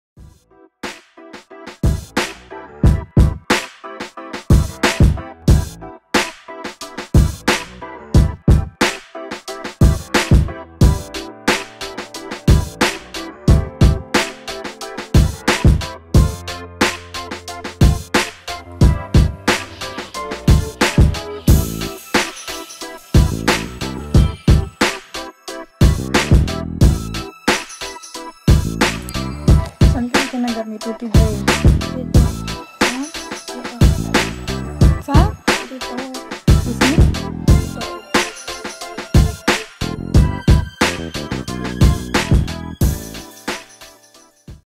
Leandre , The Frenemy of Tootie (Chihuahua)
Leandre always annoys Tootie ( that's why I captioned it Frenemy) , but in this part its Tootie's turn to annoy Leandre! 🤣😈